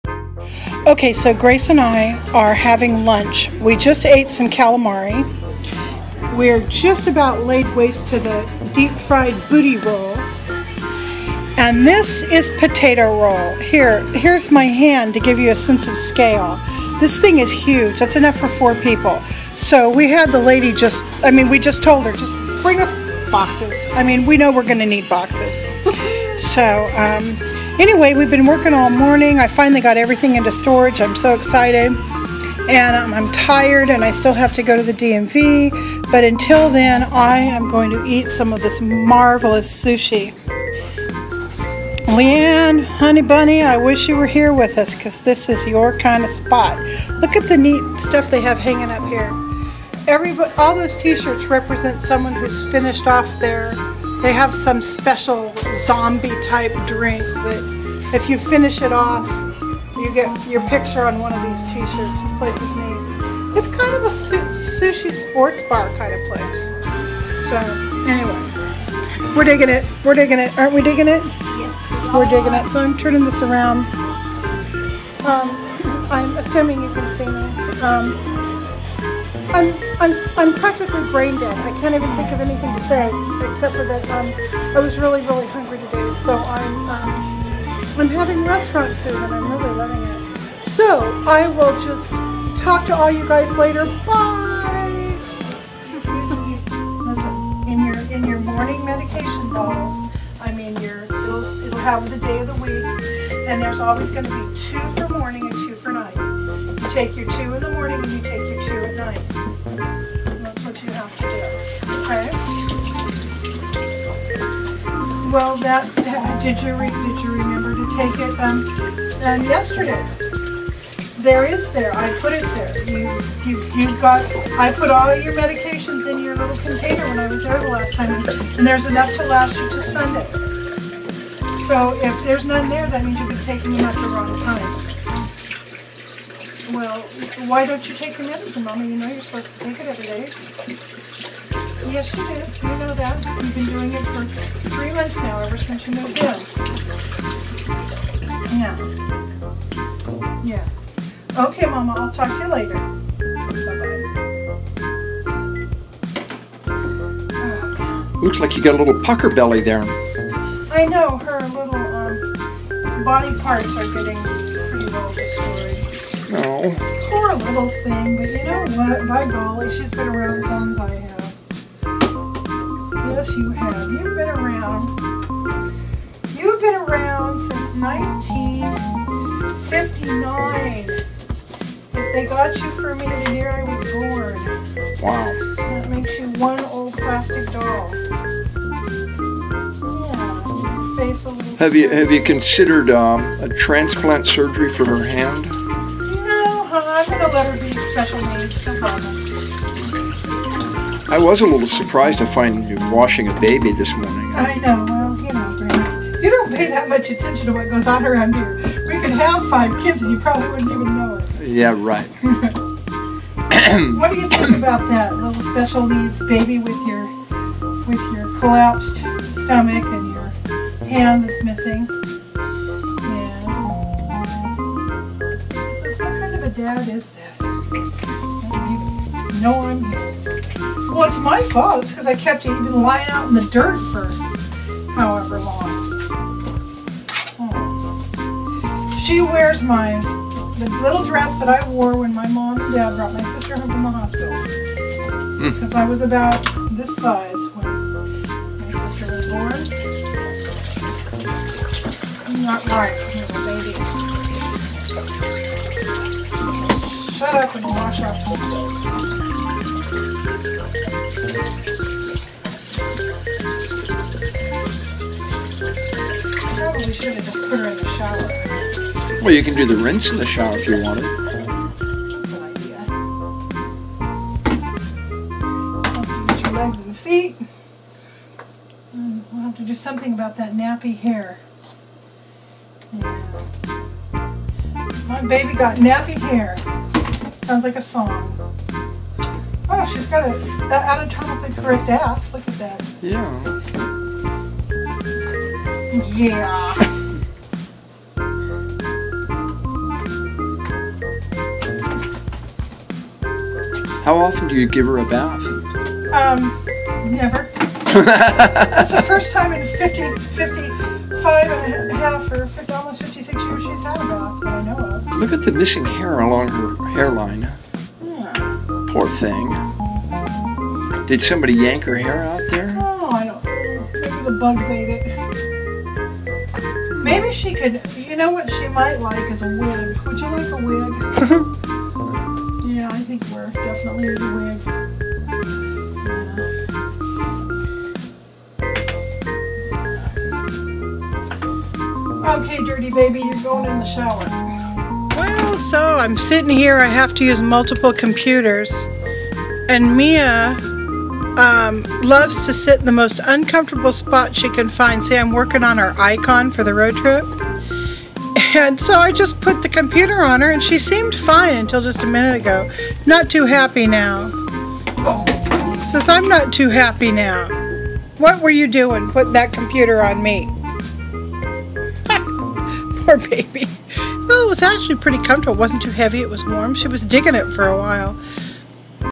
Completely Random Vlog Bits
I found these bits on my phone and, never one to waste good video footage, I thought I'd share it with you. The next video will be stop number one on the road trip - Markleeville, CA xxoo